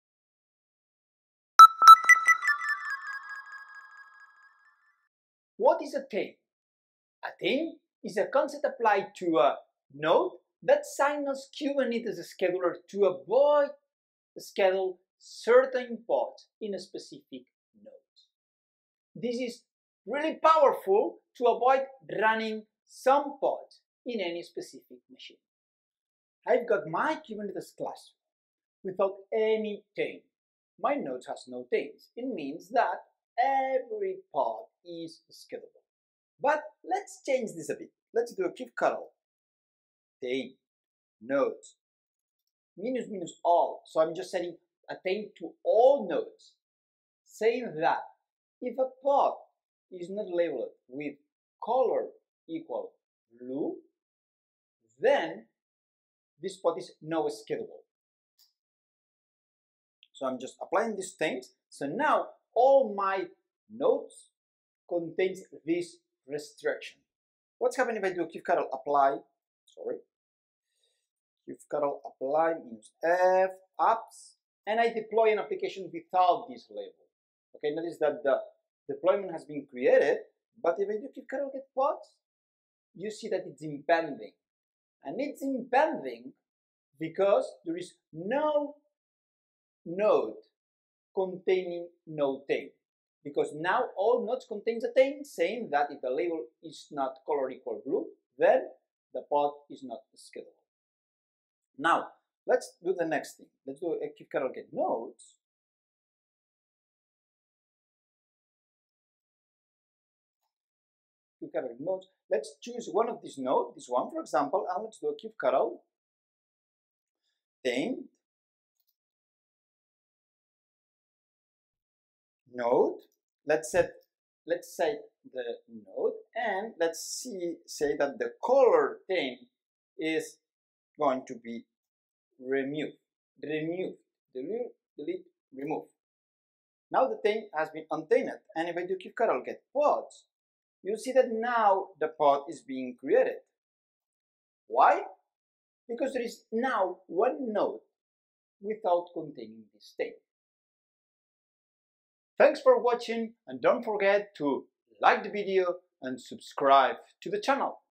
What is a Taint? | DevNation Lessons
In this DevNation Lesson, our CodeCaster Alex Soto Bueno teaches you what a Kubernetes Taint is.